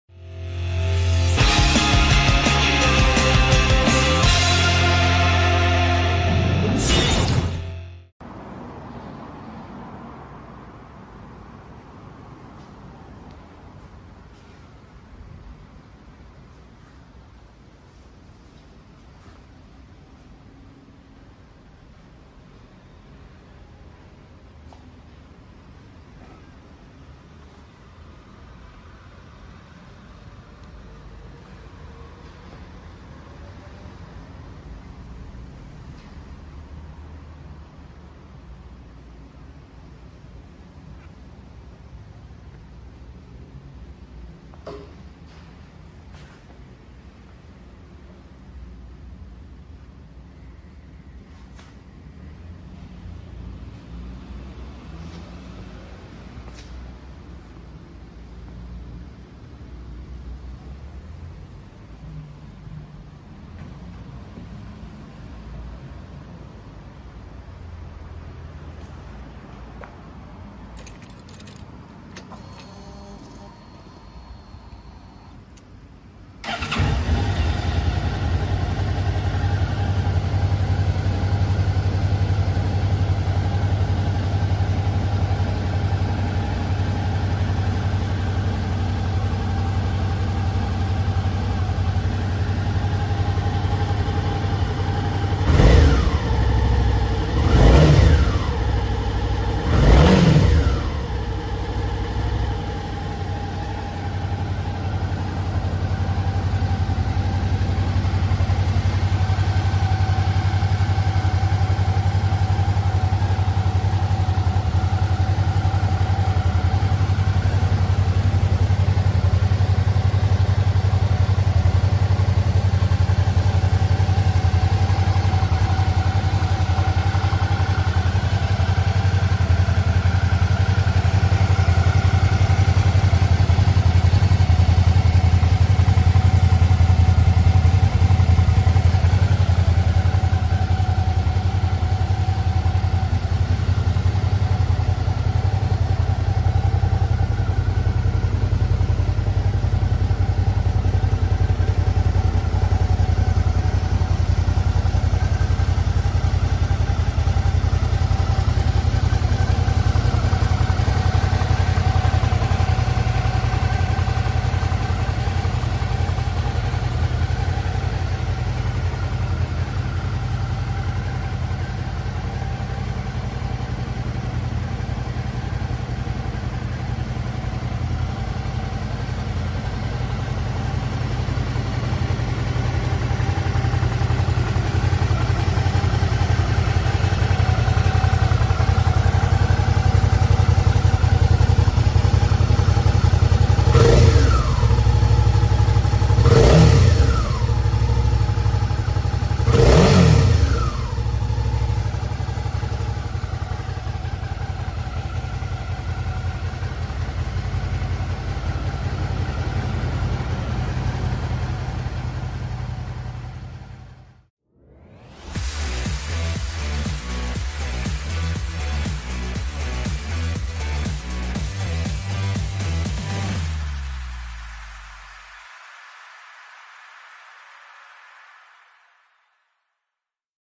スズキ SUZUKI Ｖ−ストローム６５０ＸＴ エンジン音 GIVI48Lサイドケース＋42Lトップケース！ETC2.0！LEDライト！ヘプコエンジンガード！ﾞ
各部詳細画像はこちら！